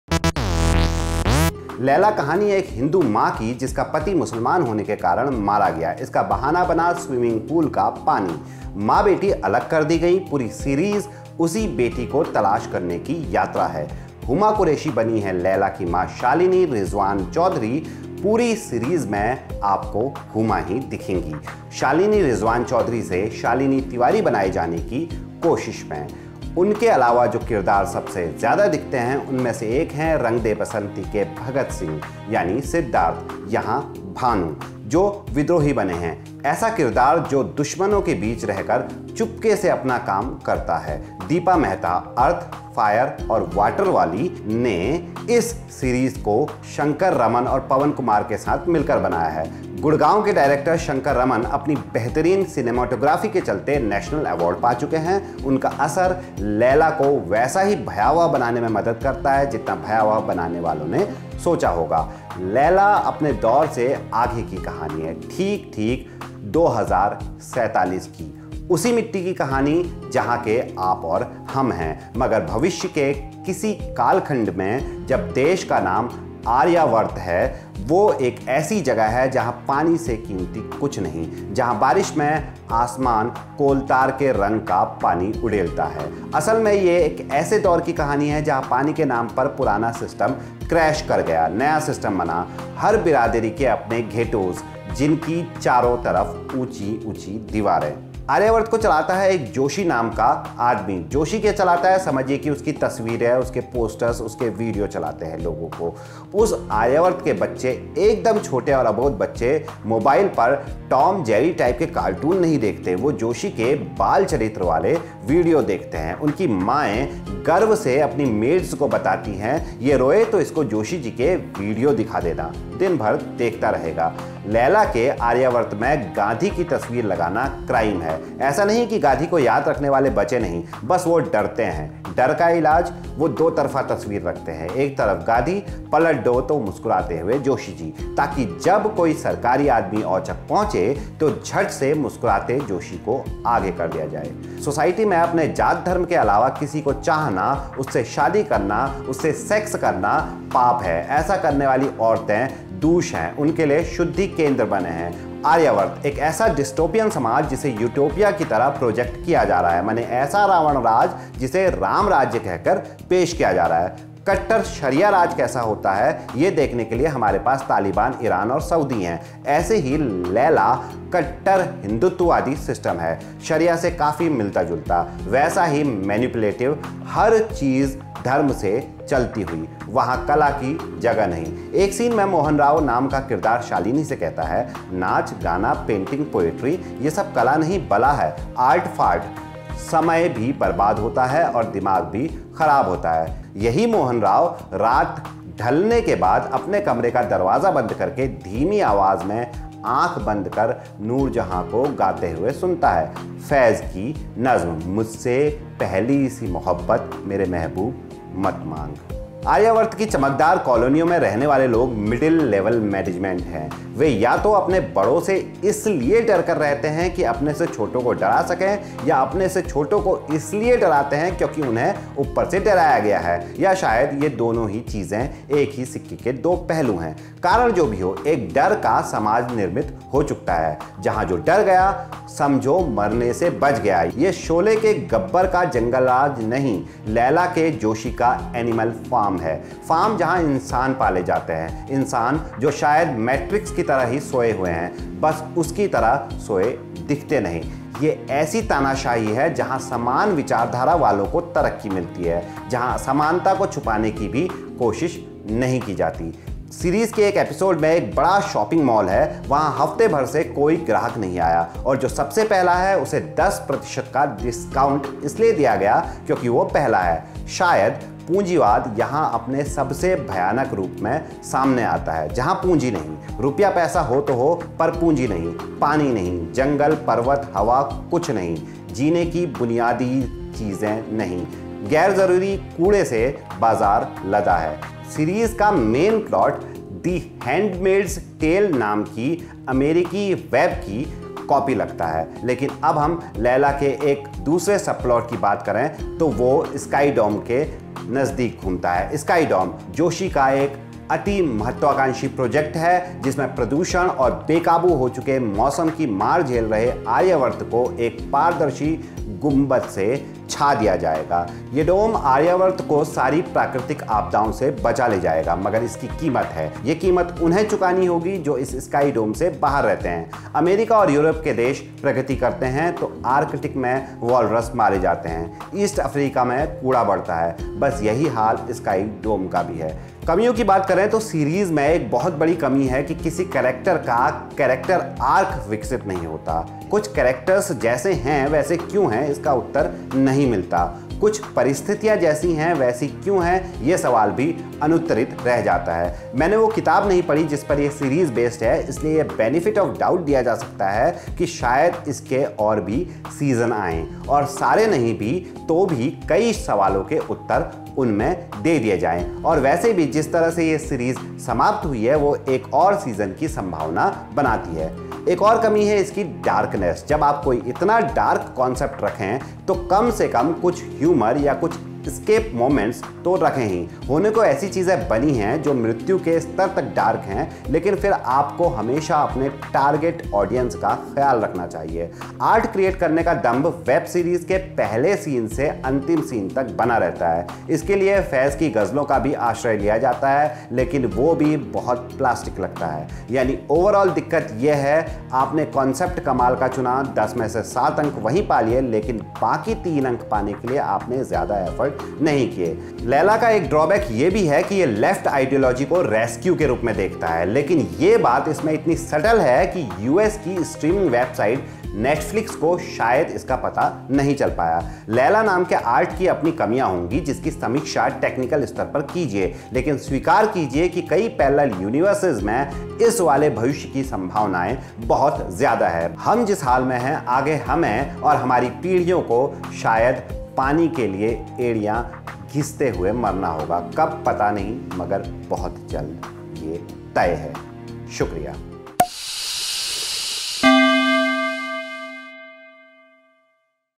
Leila: Web Series Review in Hindi | Huma Qureshi | Siddharth | Deepa Mehta | Netflix
Web Series Review in Hindi. A 6 Episode Web series, streaming on Netflix. Starring- Huma Qureshi as Shalini, Siddharth as Bhanu, Seema Biswas as Madhu, Rahul Khanna as Rizwan chaudahari,Sanjay Suri as Joshiji, Arif Zakaria as Guru Ma, Ashwath Bhatt, Pallavi Batra as Kanika, Anupam Bhattacharya, Akash Khurana, Jagjeet Sandhu as Rakesh, Prasanna Soni as Ashish. Directed by- Deepa Mehta, Shanker Raman, Pawan Kumar.